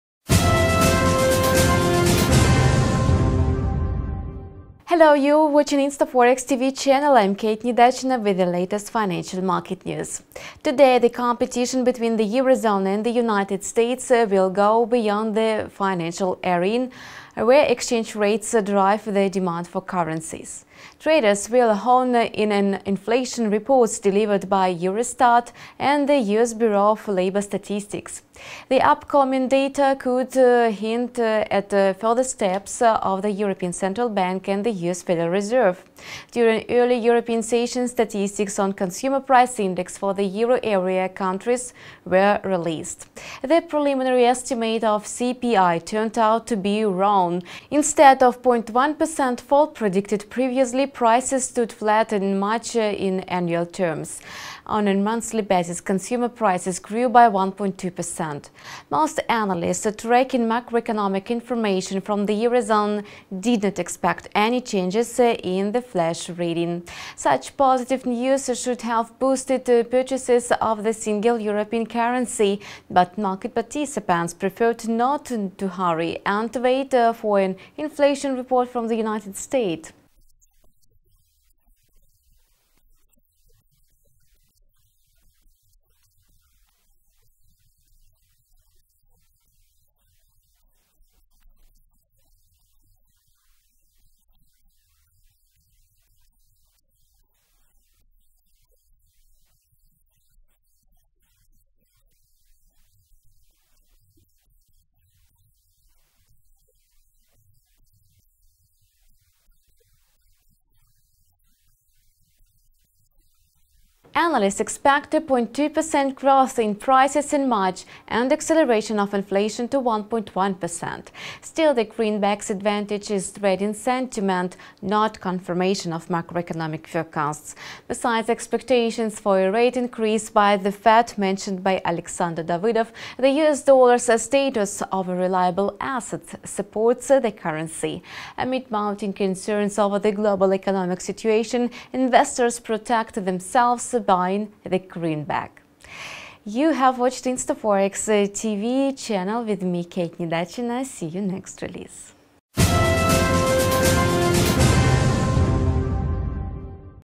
USD buyers waiting for US inflation data
Today, the competition between the eurozone and the United States will go beyond the financial arena where exchange rates drive the demand for currencies. Traders will hone in on inflation reports delivered by Eurostat and the US Bureau of Labor Statistics. The upcoming data could hint at further steps of the European Central Bank and the US Federal Reserve.
During early European session, statistics on consumer price index for the euro area countries were released.
The preliminary estimate of CPI turned out to be wrong. Instead of a 0.1% fall predicted previously, prices stood flat in March in annual terms. On a monthly basis, consumer prices grew by 1.2%. Most analysts tracking macroeconomic information from the eurozone did not expect any changes in the flash reading. Such positive news should have boosted purchases of the single European currency, but market participants preferred not to hurry and wait for an inflation report from the US.
The comment of A. Davydov:
Consumer prices in Europe stopped declining. Still, it did not help the single European currency gain ground. Now the market is awaiting inflation data release in the US. If expectations regarding inflation growth rates come true, the greenback will continue its rise. Accelerating inflation in the US boosts chances of a soon funds rate hike, and traders will bet on it. By the end of the trading day the euro could tumble to as low as 1.1200 against the US dollar.
End of comment
Analysts expect a 0.2% growth in prices in March and acceleration of inflation to 1.1%. Still, the greenback’s advantage is trading sentiment, not confirmation of macroeconomic forecasts. Besides expectations for a rate increase by the Fed, mentioned by Davydov, the US dollar’s status of a reliable asset supports the currency. Amid mounting concerns over the global economic situation, investors protect themselves buying the greenback.